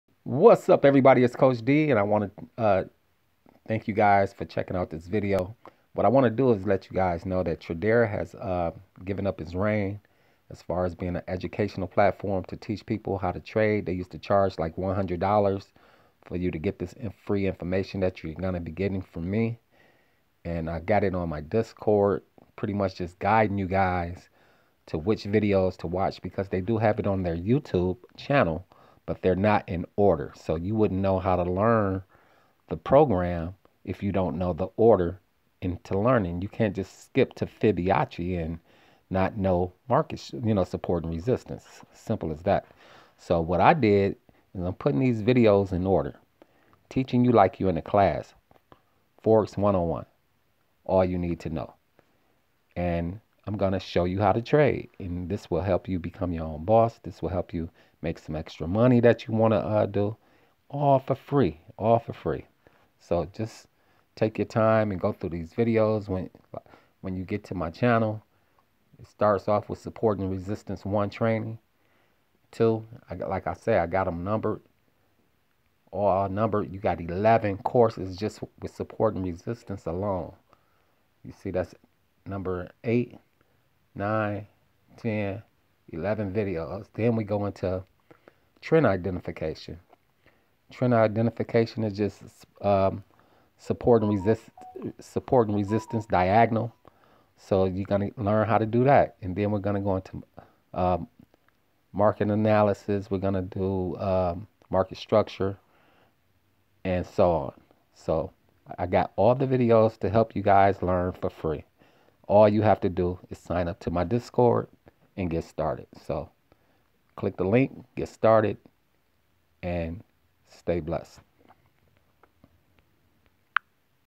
LEARN FOREX FOR FREE FULL TRADERA TRAINING IN CORRECT ORDER
On discord I will provide which videos to watch in order to learn course right. On youtube the videos are just in alphabetical order with no guide. I got you covered.

Make sure you sign up to discord. It's free